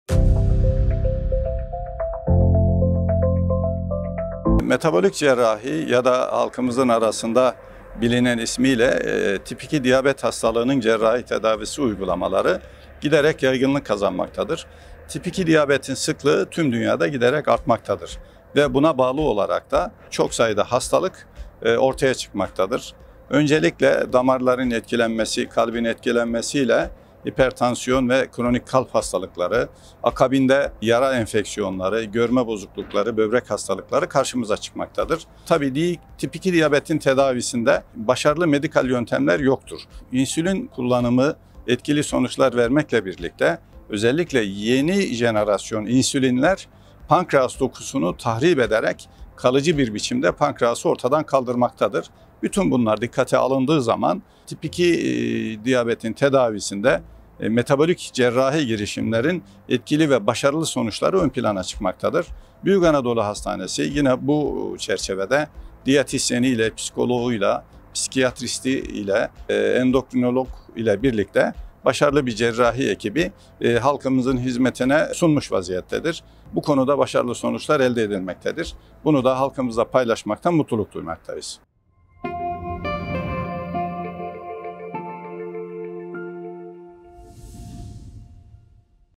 Tip 2 Diyabet Nedir ?
Genel Cerrahi Uzmanı Prof. Dr. Mustafa Şahin Anlatıyor...